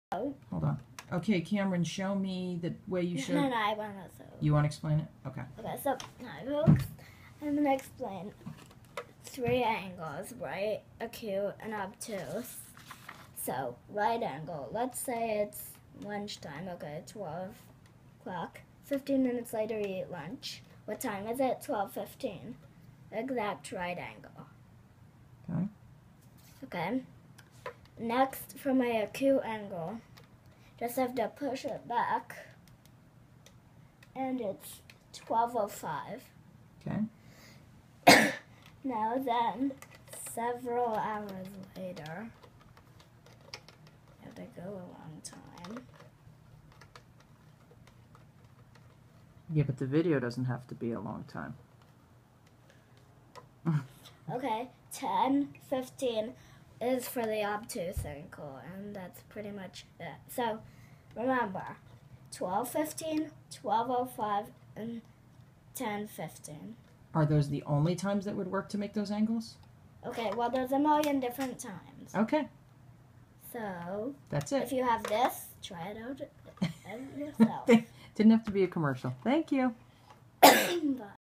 Using a clock toy to show angles
Cameron had to give three different times on an analog clock that would demonstrate right, acute, and obtuse angles. Luckily, he had this tool from when we learned to tell time.